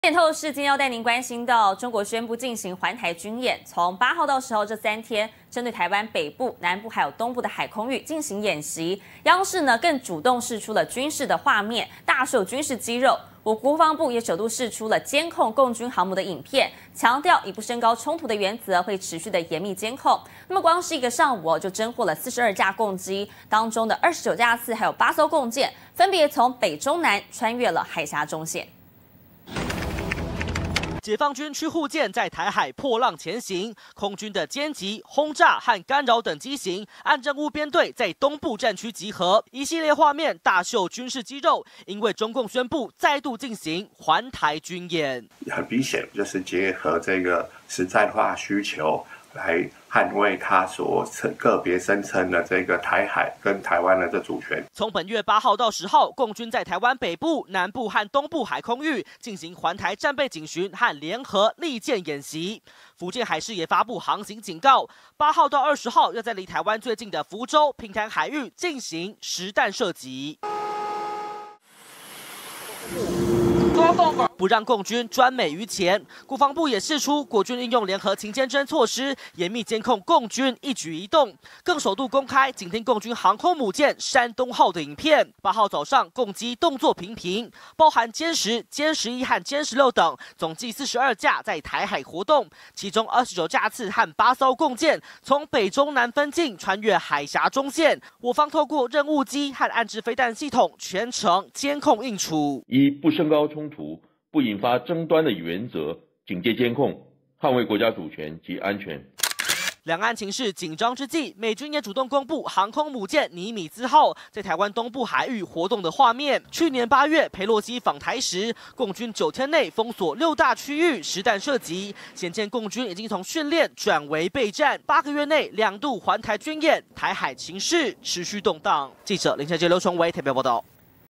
國防部嚴密監控共軍! 首公開緊盯山東號影片
中國宣布進行環台軍演，8號到10號這三天，針對台灣北部、南部和東部海空域，進行演習，央視更主動釋出軍演畫面，大秀軍事肌肉。我國國防部也首度釋出山東號監控共軍航母的影片，強調以不升高衝突的原則，會持續的嚴密監控。光是一個上午，就偵獲42架共機，其中29架次、8艘共艦，分別從北、中、南，穿越海峽中線。